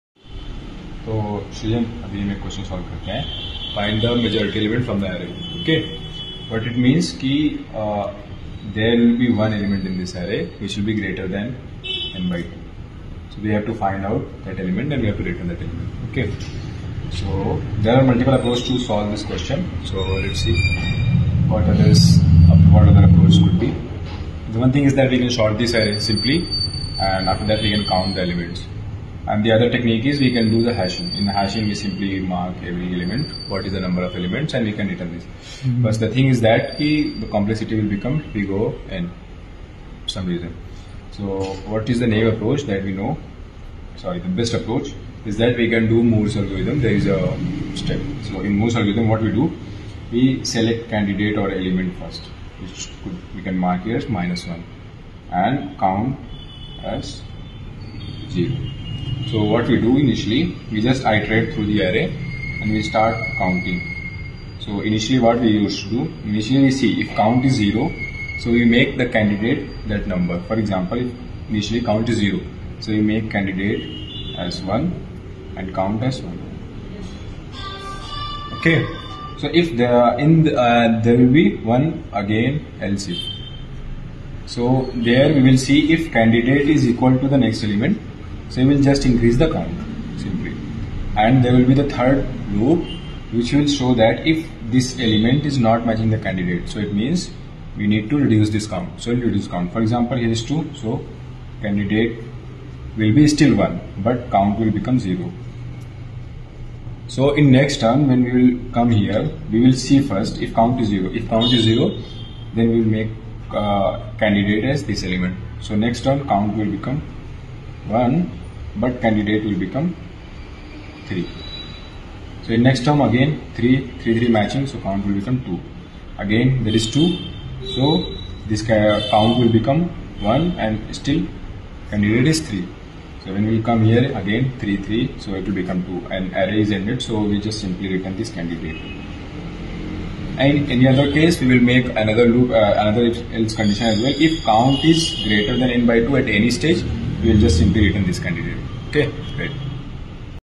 Boyer-Moore Algorithm Explained with Code | Majority Element
In this video, we solve the classic coding interview problem: Find the Majority Element in an Array.

This is a popular LeetCode problem (#169) that’s frequently asked in technical interviews. I explain the Boyer-Moore Majority Voting Algorithm and show how to implement it efficiently.

Problem Statement:
Given an array of size n, return the element that appears more than ⌊n / 2⌋ times.

Approach:
We use the Boyer-Moore Voting Algorithm which works in O(n) time and O(1) space.